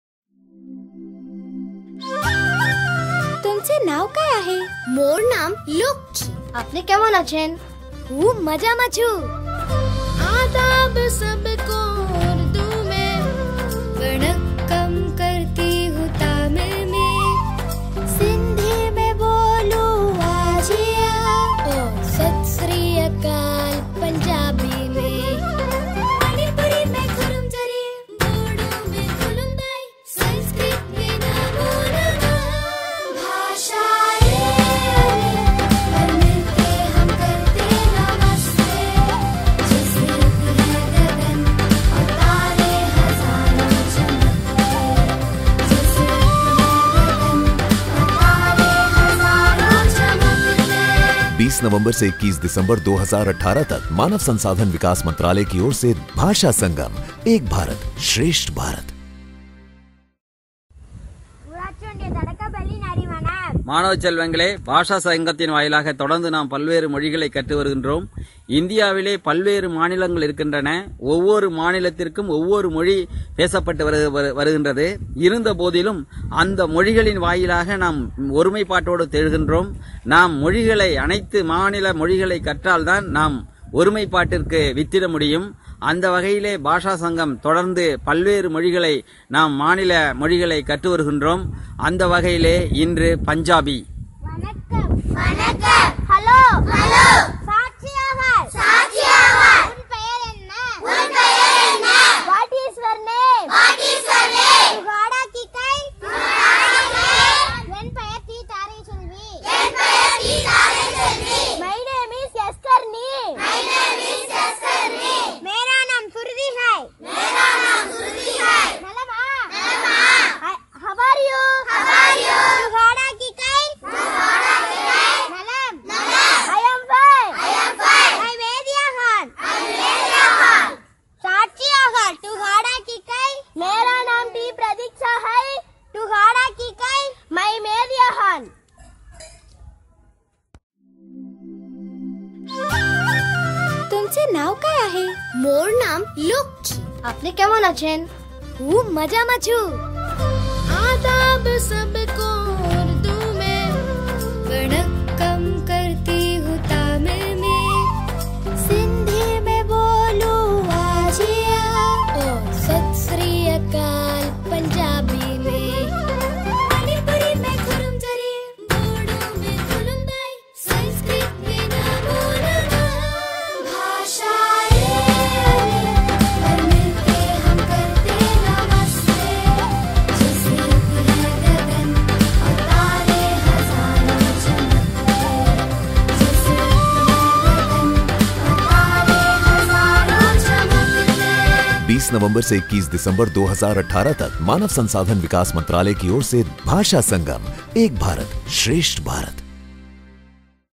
BHASHA SANGAM - PUPS NARIMANAM PUNJABI
LANGUAGE : PUNJABI
 NAME OF THE SCHOOL : PANCHAYAT UNION PRIMARY SCHOOL NARIMANAM
 FULL ADRESS : PANCHAYAT UNION PRIMARY SCHOOL NARIMANAM
THIRUMARUGAL BLOCK
NAGAPATTINAM DISTRICT
TAMIL NADU
DETAILS OF THE VIDEO : BHASHA SANGAM - PANCHAYAT UNION PRIMARY SCHOOL NARIMANAM ( PUNJABI)